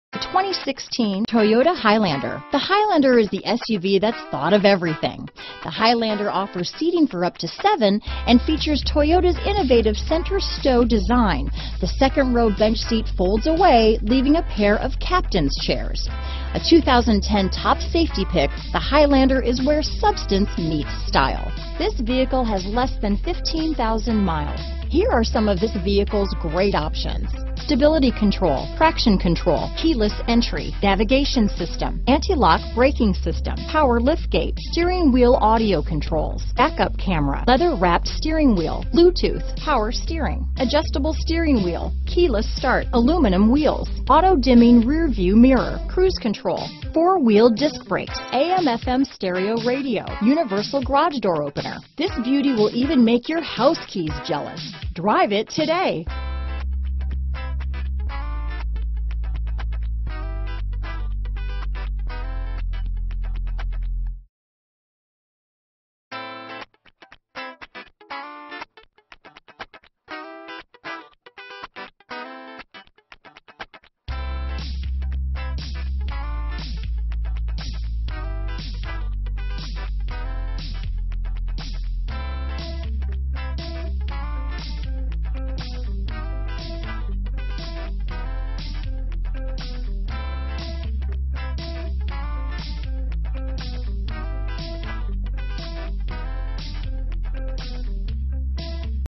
2016 Toyota Highlander Lakeland, Plant City, Winter Haven, FL J0Z17763TA
XLE trim. EPA 25 MPG Hwy/19 MPG City! ONLY 14,022 Miles!